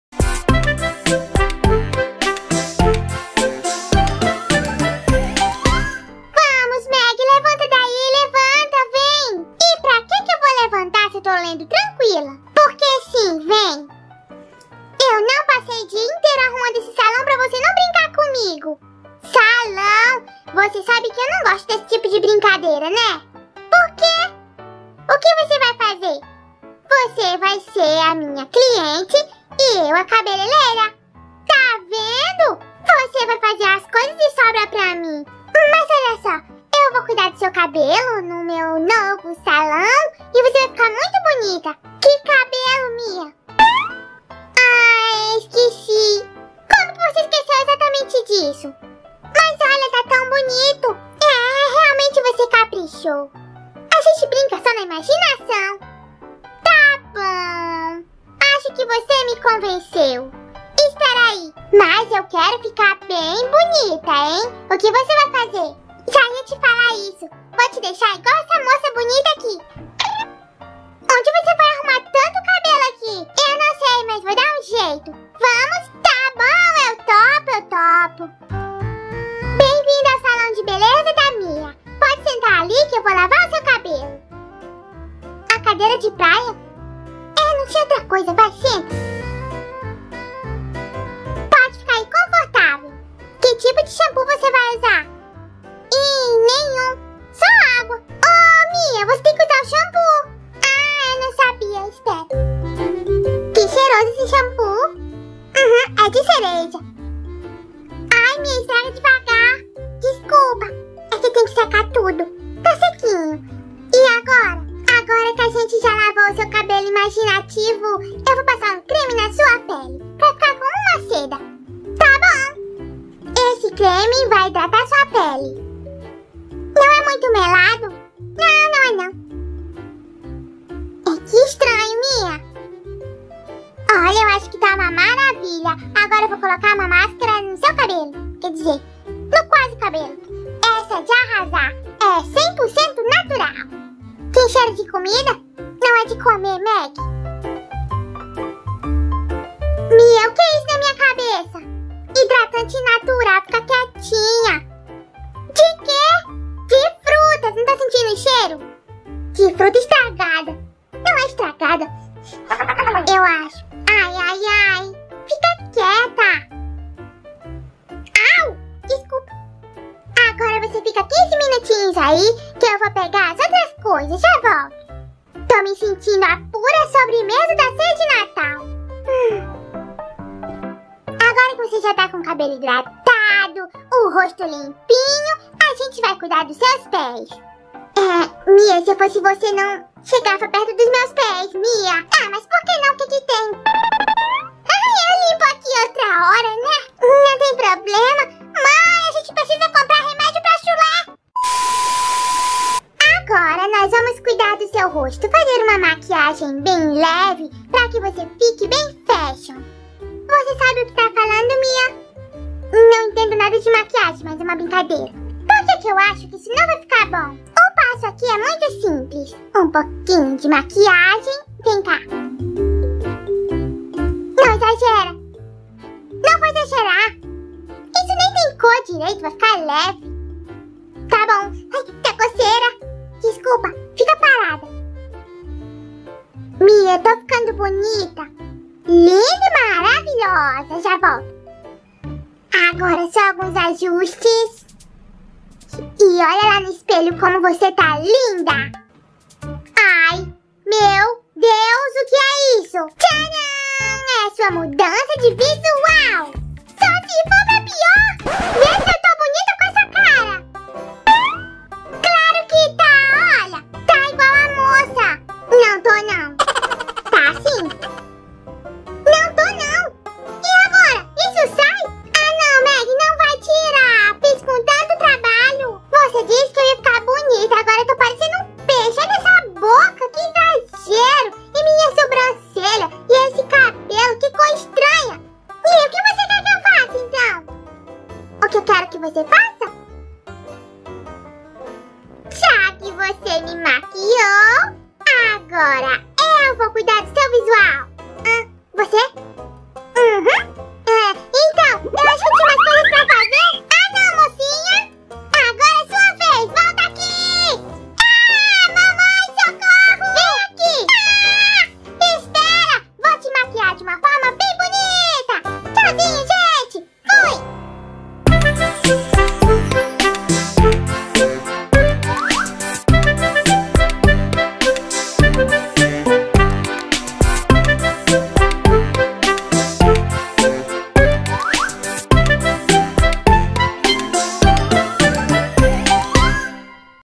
O SALÃO DE BELEZA DA MIA! - Lilly Doll
Acompanhe mais essa divertida novelinha de bonecas para crianças.

CAIXA POSTAL 34
Horizonte- CE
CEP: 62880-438

Não esqueça de deixar o seu like e se inscrever!!